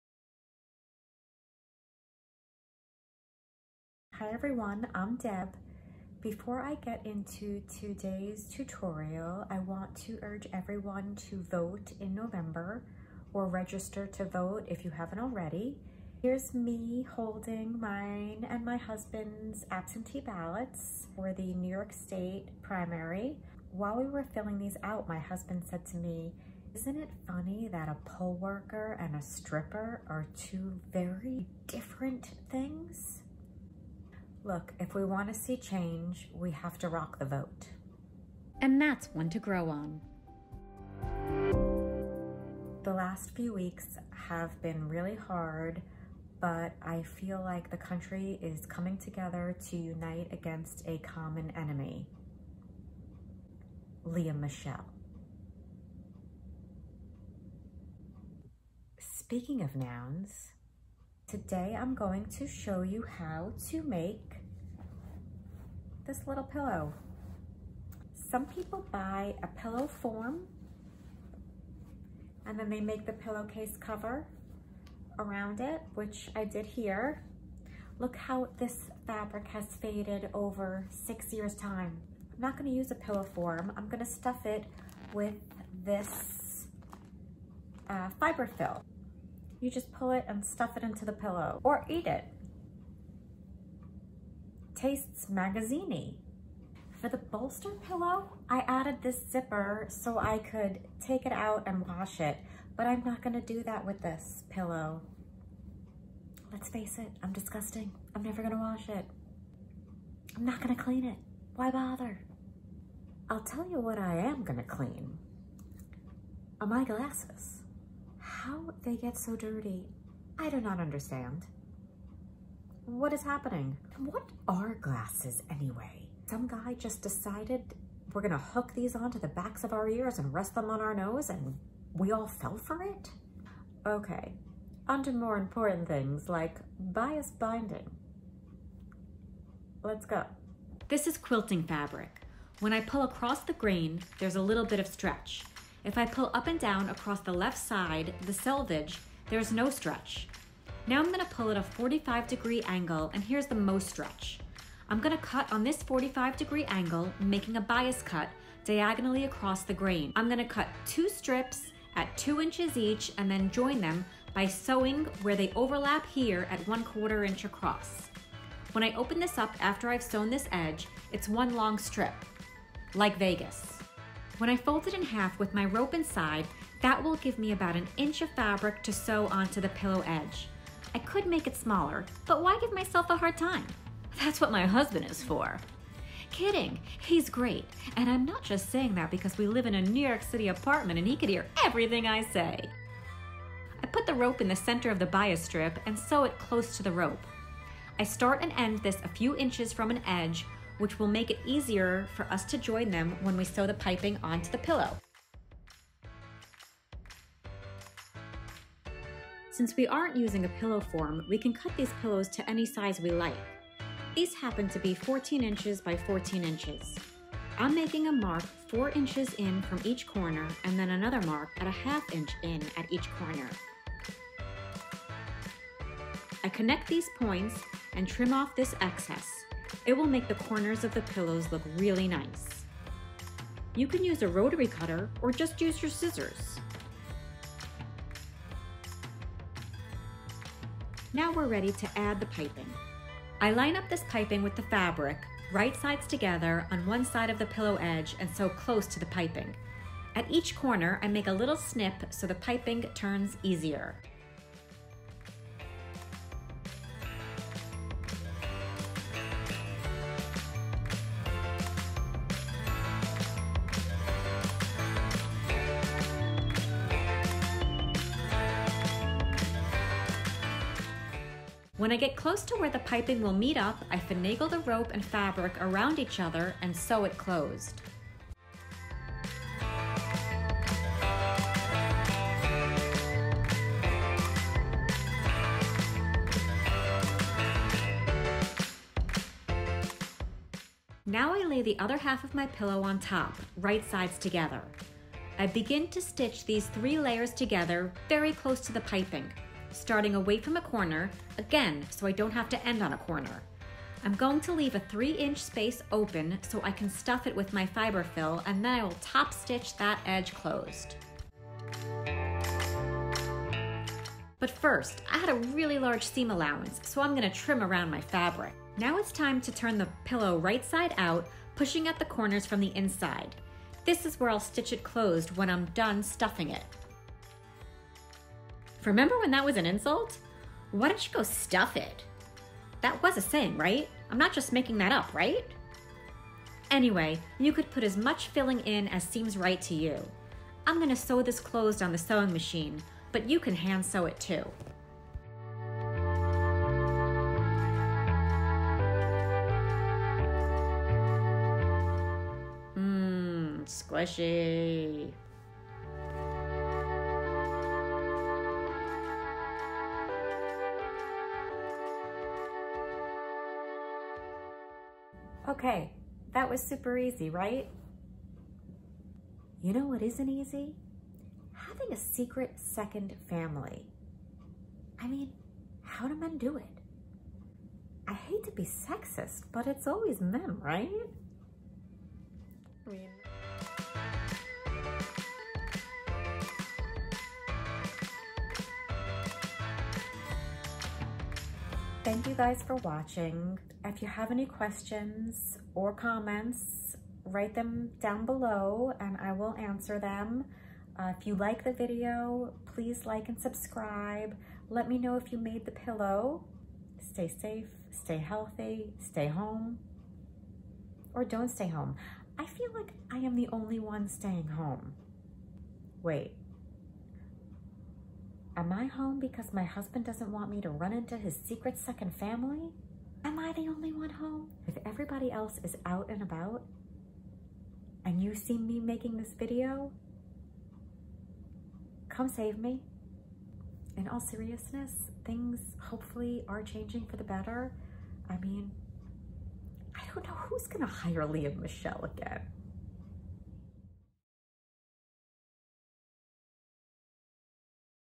How to Make Awesome Pillows! Custom Size, Piping, Bias Binding
Pillows are what separate us from the animals. Now, you can make your own—in any size—with piping and bias binding.